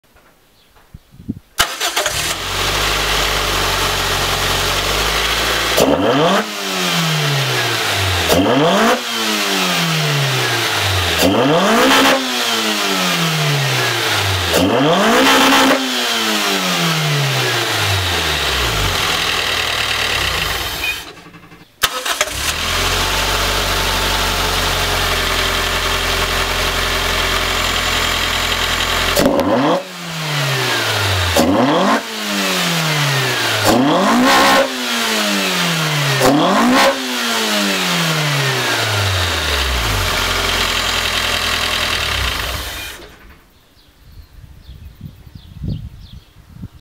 4AGE 20v Blacktop 110mm Open Trumpets At Full Throttle
Just installed some 110mm trumpets (ram tubes/velocity stacks) on my 4A-GE 20-valve Blacktop engine. Open trumpets, no filtration. Engine is in a 1993 Toyota Corolla SE Limited (AE100, JDM).